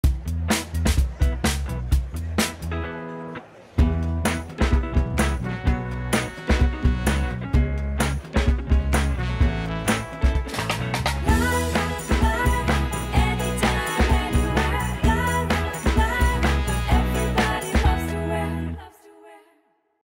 Lyra Club TVC_2022 Ft. Taapsee Pannu
Look effortlessly chic and playful with wide colorful range of Lyra Bottom Wear.